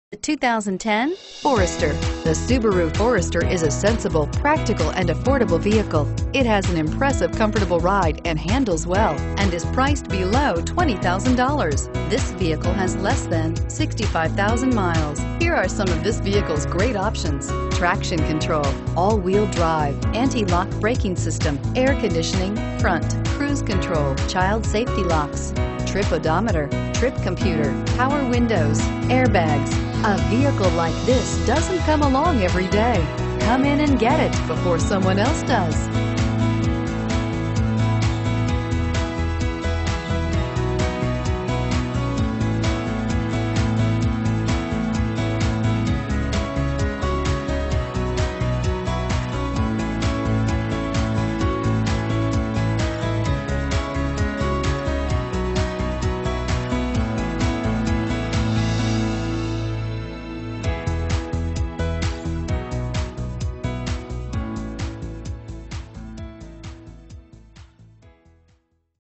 2010 Subaru Forester Burlington WA B8152A
2010 Subaru Forester 2.5X

Sims Honda
1615 S. Goldenrod Road

CLEAN CARFAX, LOCAL TRADE. !, And ROOF RAILS.